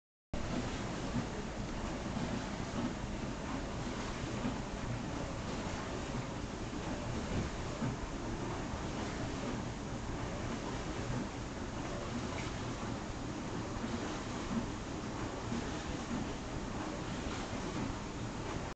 Rolling Funny Cat
Hopps doing his awesome rollie thing